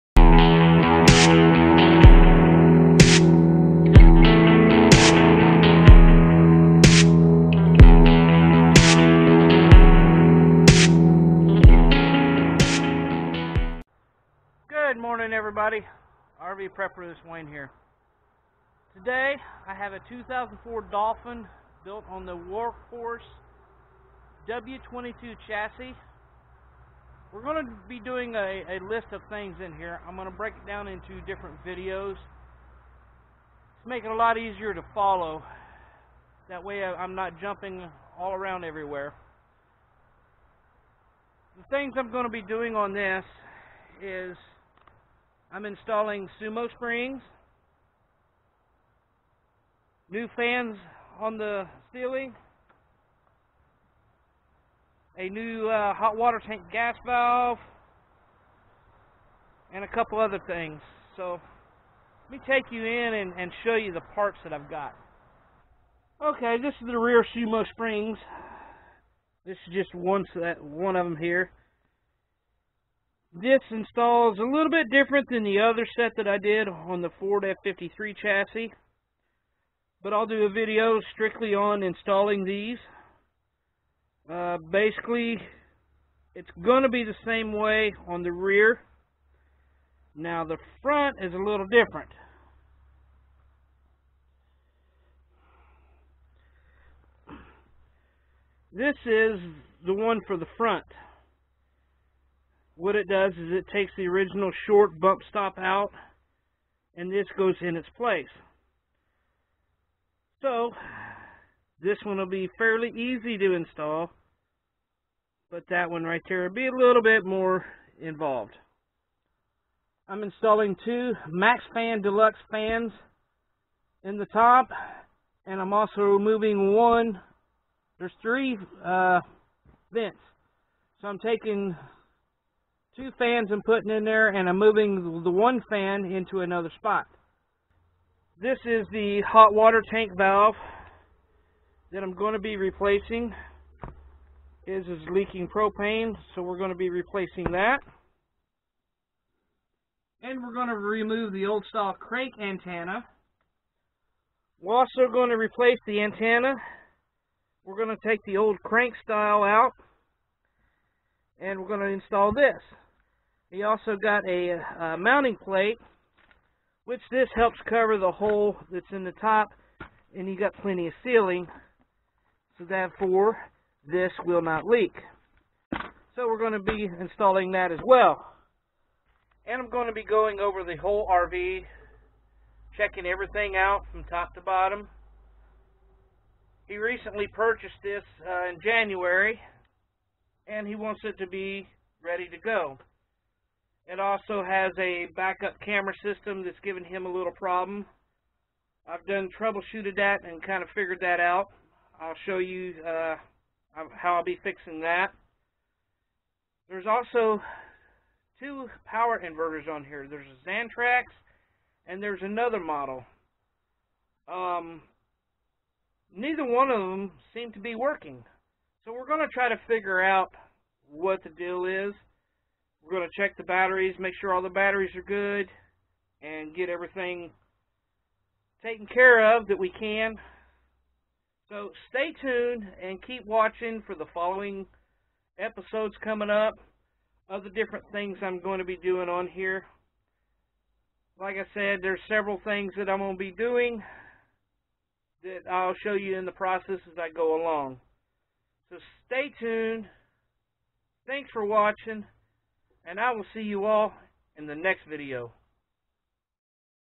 2004 Dolphin Repair list
I have a 2004 National RV Dolphin that I have a list of repairs and upgrades.